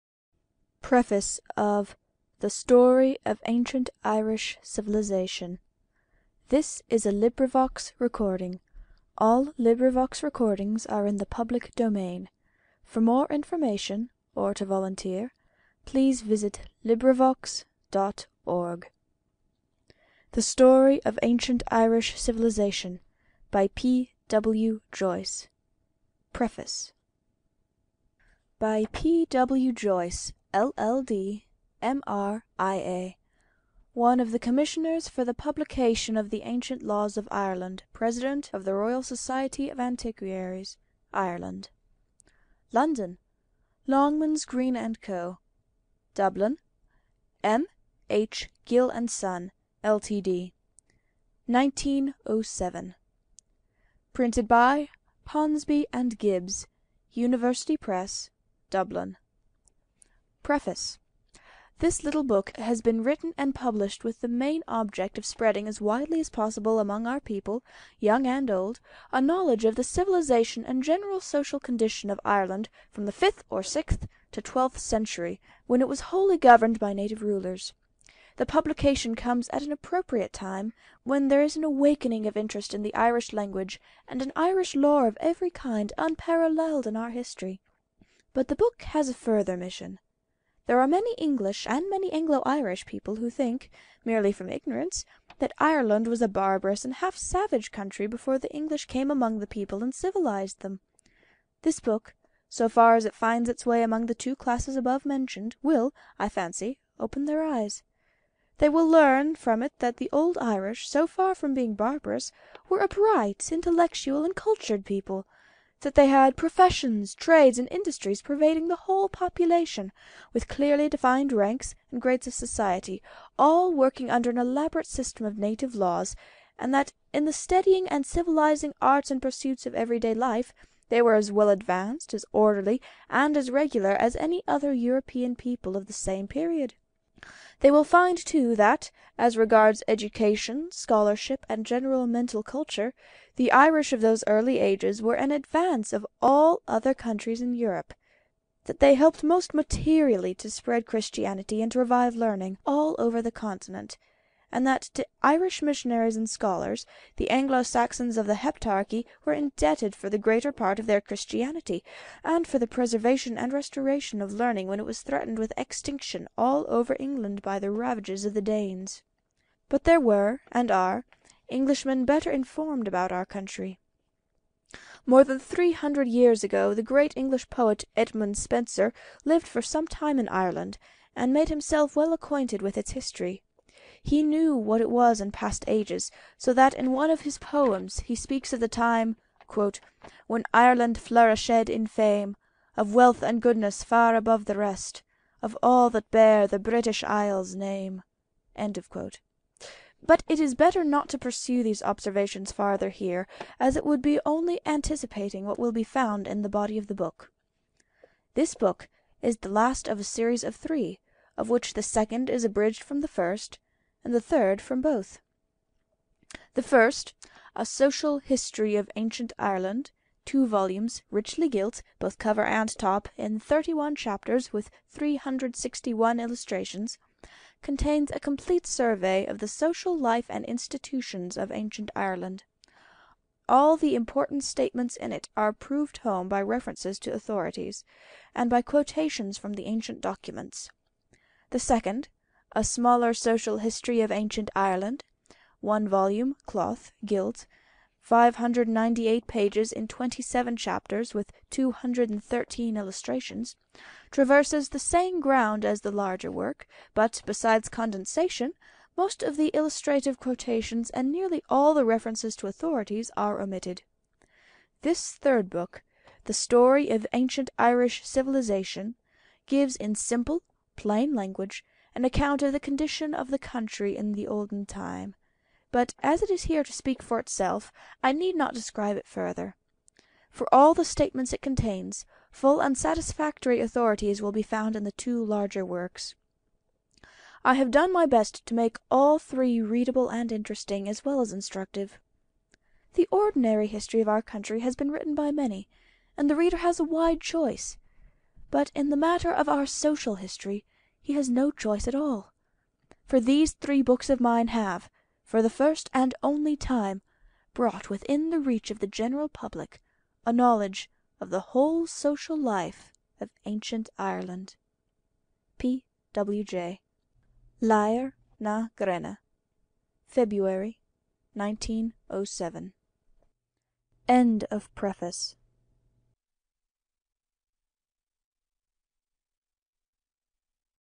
0 preface
THE STORY OF ANCIENT IRISH CIVILIZATION.
This little book has been written and published with the main object of spreading as widely as possible among our people, young and old, a knowledge of the civilisation and general social condition of Ireland from the fifth or sixth to the twelfth century, when it was wholly governed by native rulers. The publication comes at an appropriate time, when there is an awakening of interest in the Irish language, and in Irish lore of every kind, unparalleled in our history.

AUTHOR BIOGRAPHY.
Patrick Weston Joyce, known as P. W. Joyce (1827 – January 7, 1914) was an Irish historian, writer and music collector, known particularly for his research in Irish etymology and local place names of Ireland. He was born in Ballyorgan in the Ballyhoura Mountains, on the borders of counties Limerick and Cork in Ireland, and grew up in nearby Glenosheen. The family claimed descent from one Seán Mór Seoighe (fl. 1680), a stonemason from Connemara, County Galway. Joyce started work in 1845 with the Commission of National Education. He became a teacher and principal of the Model School, Clonmel. In 1856 he was one of fifteen teachers selected to re-organize the national school system in Ireland. Meanwhile he earned his B.A. in 1861 and M.A. in 1863 from Trinity College, Dublin. He was principal of the Training College, Marlborough Street, in Dublin from 1874 to 1893. As a member of the Society for the Preservation of the Irish Language he wrote an Irish Grammar in 1878. He was President of the Royal Society of Antiquaries of Ireland from 1906 to 1908, an association of which he was a member from 1865.

Joyce was a key cultural figure of his time. His wide interests included the Irish language, Hiberno-English, music, education, Irish literature and folklore, Irish history and antiquities, place-names and much else. He produced many works on the history and culture of Ireland. His most enduring work is the pioneering The Origin and History of Irish Names of Places (first edition published in 1869).[2] He was a member of the Royal Irish Academy.